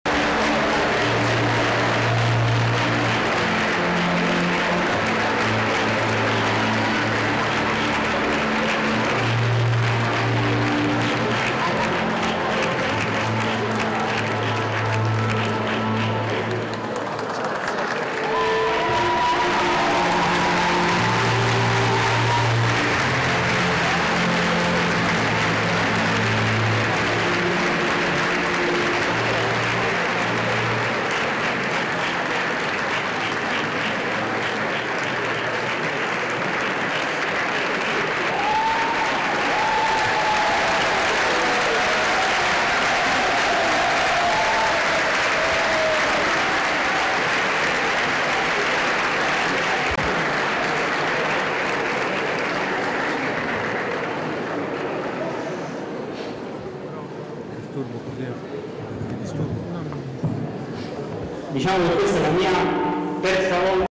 L'arrivo di Papa Francesco agli Stati Generali della Famiglia
(Agenzia Vista) - Roma, 10 Maggio 2024 - Papa Francesco è intervenuto agli Stati Generali della Famiglia. All'entrata in sala, presso l'Auditorium Conciliazione, il Pontefice è stato accolto da molti applausi. Sul palco il Papa ha stretto la mano a una bambina.